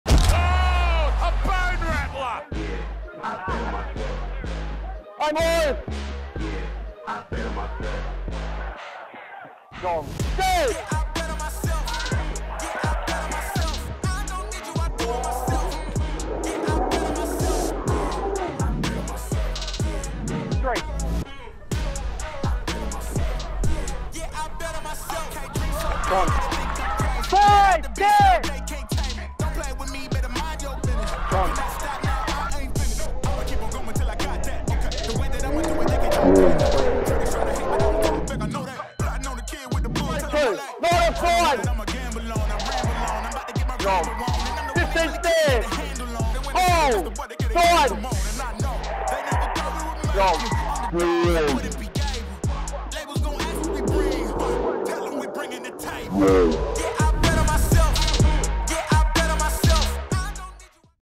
QRL Crunch💥 Which do you think is the biggest hit... comment A, B or C?
Time to vote for the first QRL Crunch of '24 - comment A, B or C for your hit of the week. Comment your favourite from...
A. Tavake Tau'a'alo (Tweed Seagulls / Cyril Connell Cup)
B. Hawke Kahu (Souths Logan Magpies / Harvey Norman Under 17s)
C. Marino Rimene (Wynnum Manly Seagulls / Cyril Connell Cup)